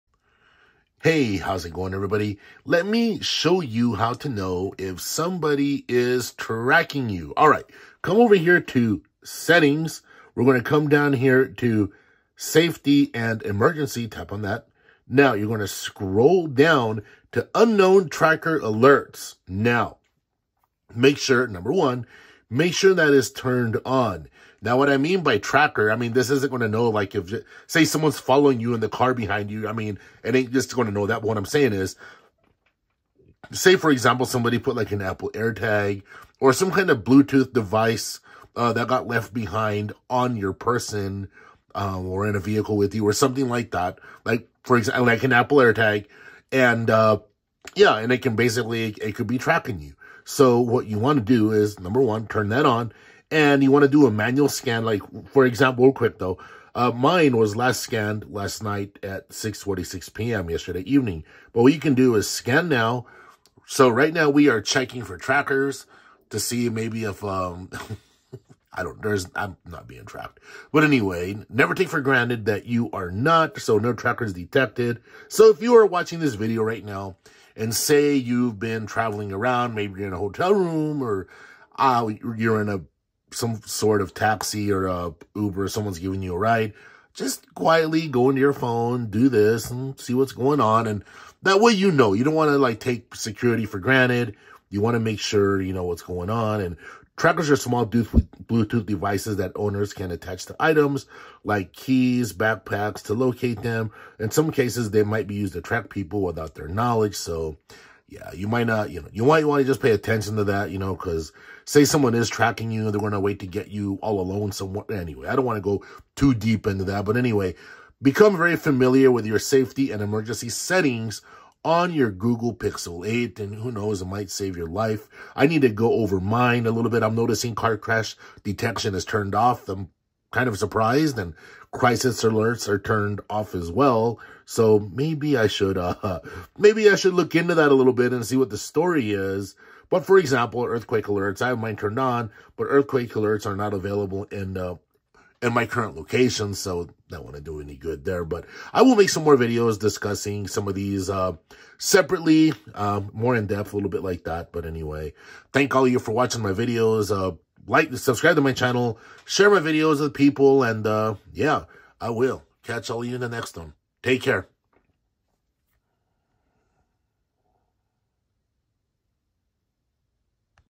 Is somebody tracking your movements? Here's what you need to know, Google Pixel 8
in this video, I'm going to show you how you can use your Google Pixel 8 to scan for possible tracking devices.

stay safe everybody.